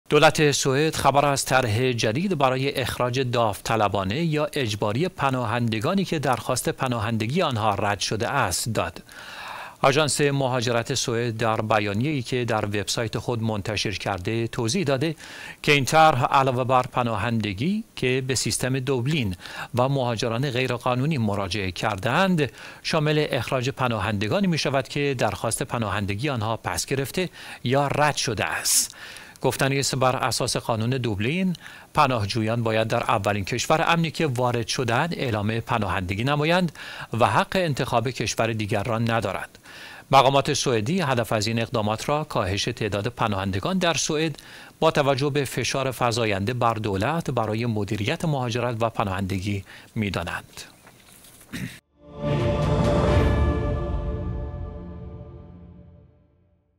سوئد طرح اخراج هزاران پناهنده را کلید زد؛ سرنوشت نامعلوم برای پناهجویان رد شده!
طرح اخراج پناهندگان سوئد به تازگی در دستور کار دولت این کشور قرار گرفته است. این طرح شامل اخراج داوطلبانه یا اجباری پناهندگانی است که درخواست پناهندگی آن‌ها رد یا پس گرفته شده است.
سوئد با افزایش فشار برای مدیریت مهاجرت و پناهندگان، هدف از اجرای این سیاست را کاهش تعداد مهاجران و بازگشت به یک سیستم پایدارتر می‌داند.
 بر اساس این طرح، افرادی که مطابق قانون دوبلین وارد کشور شده‌اند و یا به‌صورت غیرقانونی اقامت داشته‌اند نیز در لیست اخراج قرار می‌گیرند.

اما چه پیامدهایی برای جامعه پناهندگان خواهد داشت؟ آیا این سیاست می‌تواند باعث تشدید بحران‌های انسانی در دیگر کشورها شود؟
 برای دریافت اطلاعات بیشتر و پاسخ به این سوالات، حتماً ویدیو کامل را مشاهده کنید و به تحلیل‌هایی که درباره طرح اخراج پناهندگان سوئد ارائه شده، توجه کنید.

ویدیوهای مرتبط

ویدیوهای مرتبط